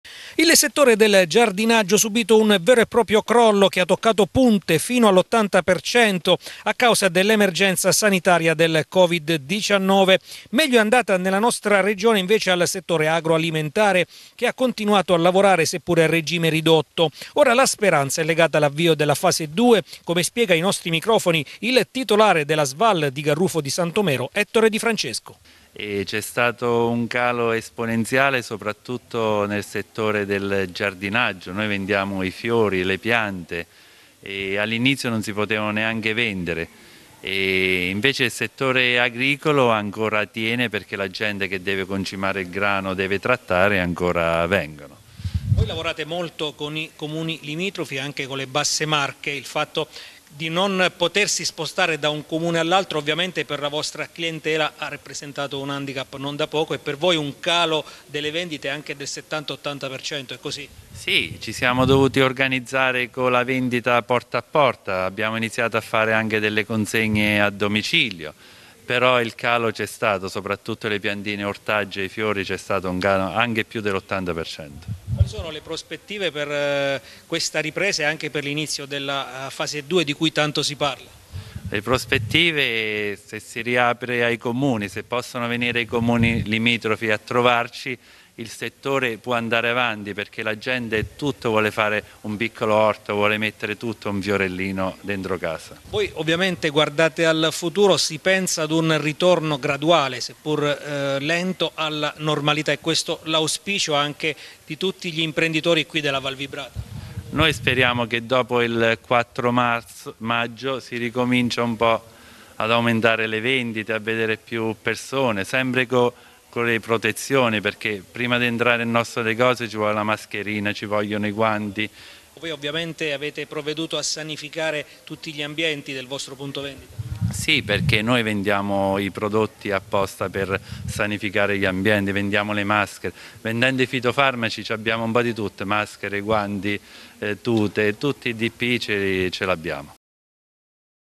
Giardinaggio. Crollo delle vendite fino al 80%
Fra i settori che hanno risentito di più dell'emergenza sanitaria, c'è sicuramente quello del giardinaggio che ha avuto un pesante crollo nella nostra regione